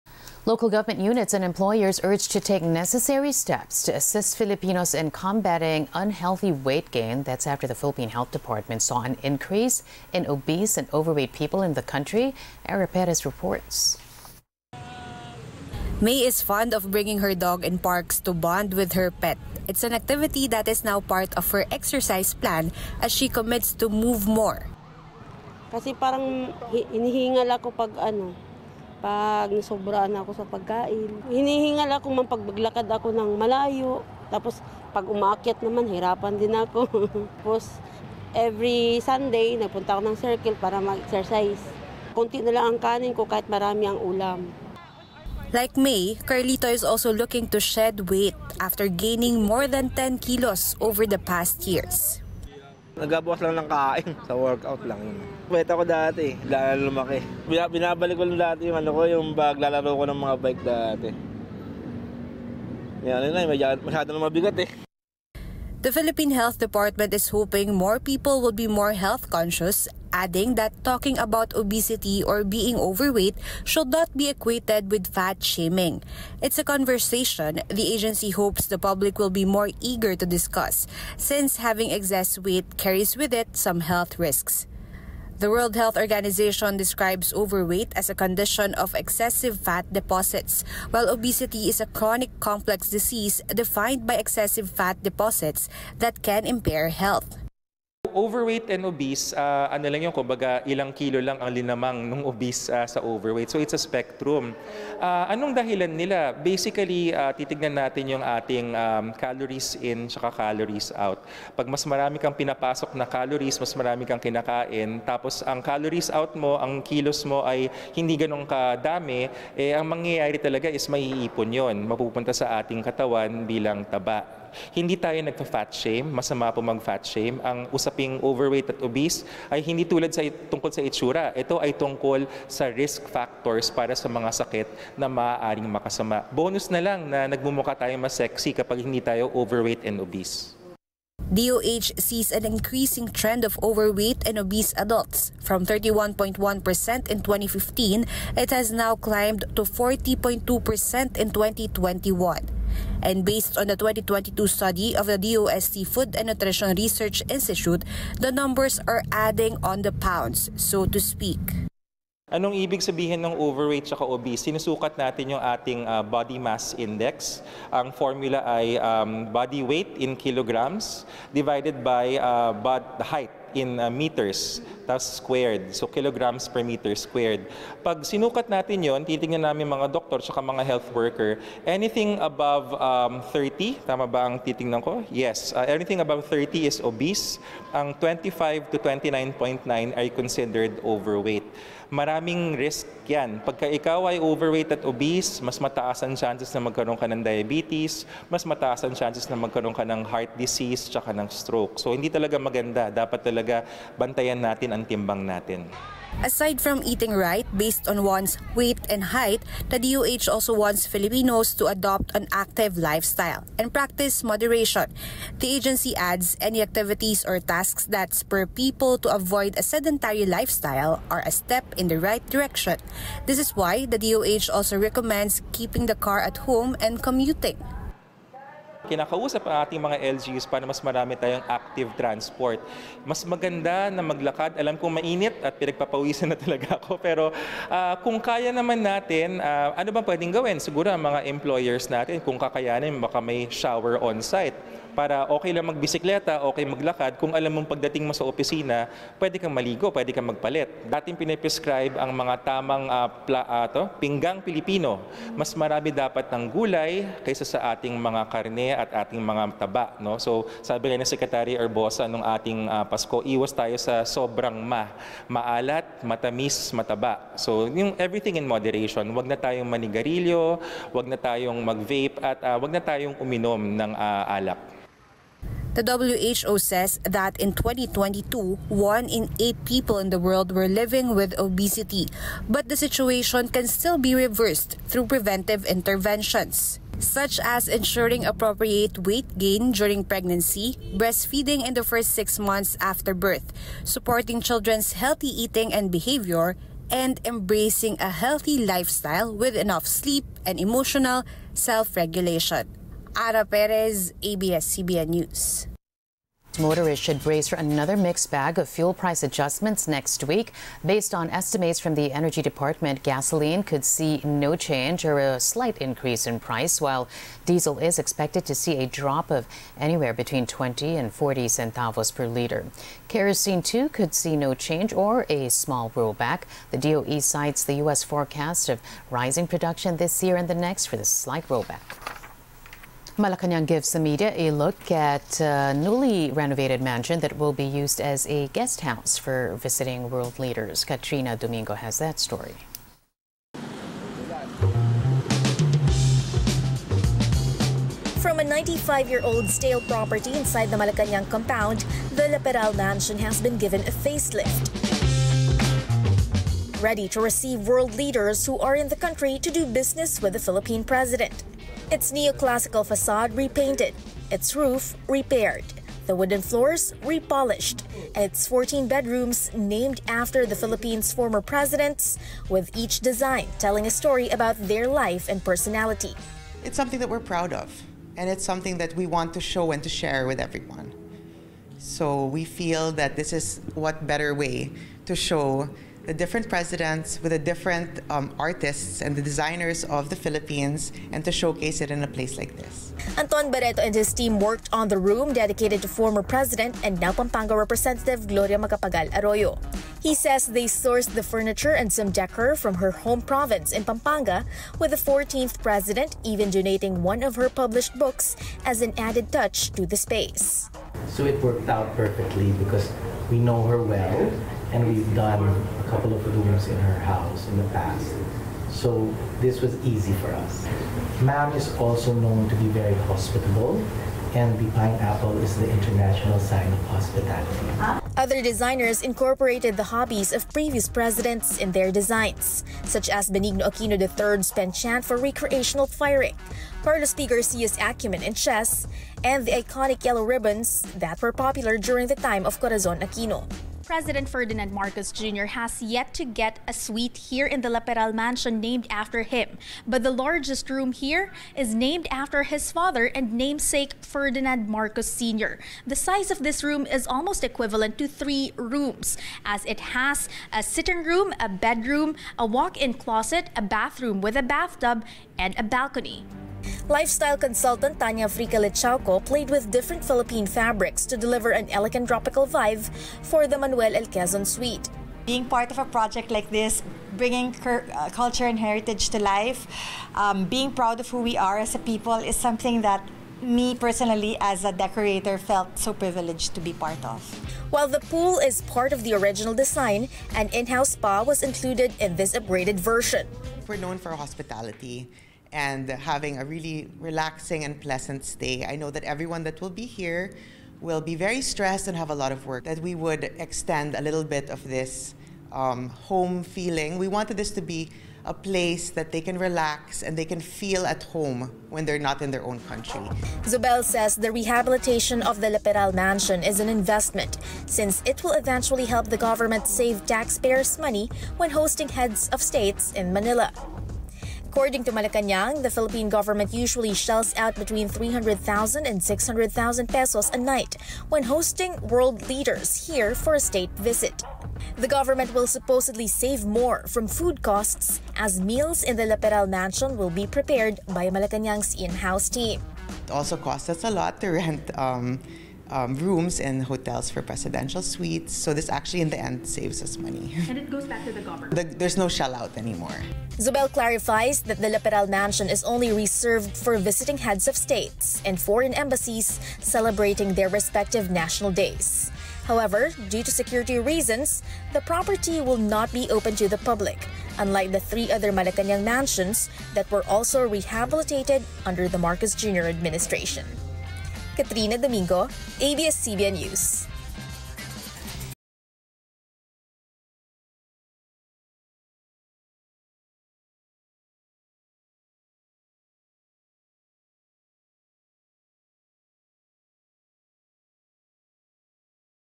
WHO: One in eight people in the world living with obesity | ANC
Local government units and employers are urged to take necessary steps to assist Filipinos in combating unhealthy weight gain.